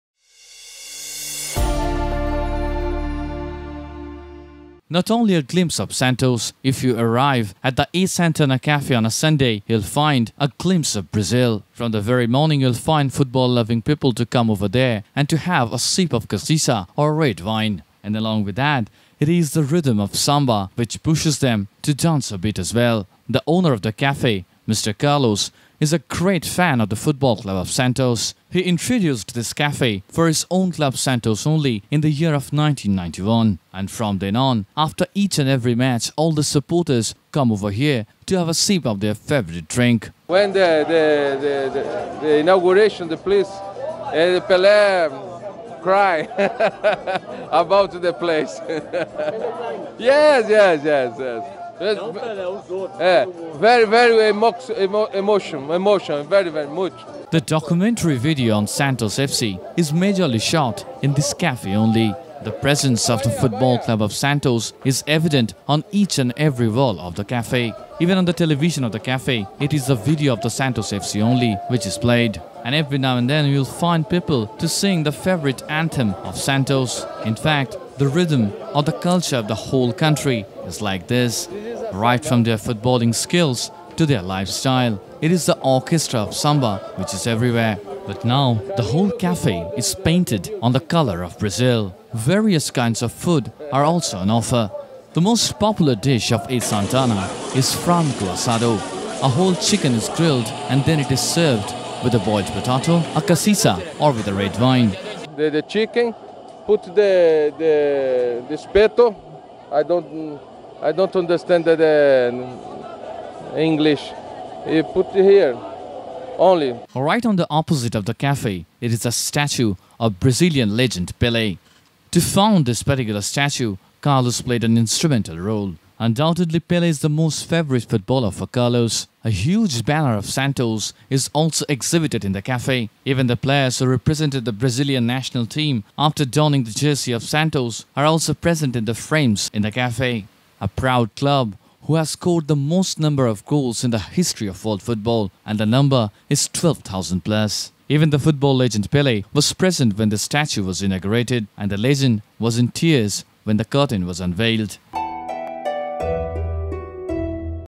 A Santana:A trip to Brazilian football
There is a special cafe is Santos which has an aura of Samba and soccer.Find out more about this special cafe.